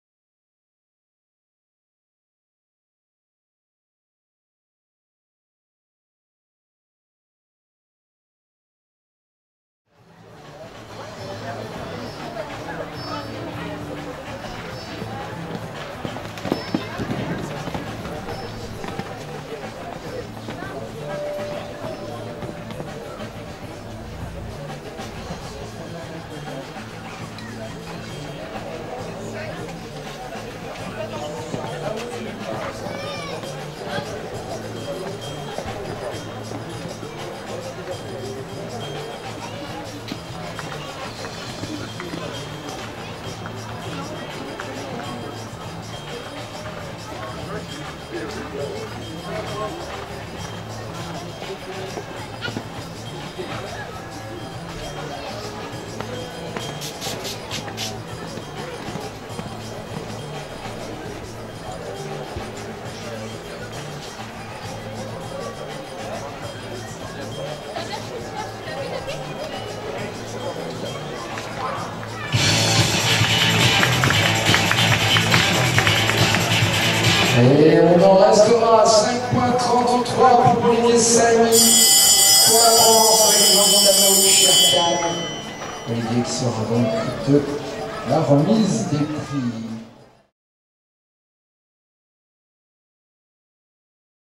VTS 01 1 LANTANO SHERKHAN
Finale Critérium AM1 Séniors - Hauteur 120 cm - Le Mans Boulerie Jump - 02 Août 2009...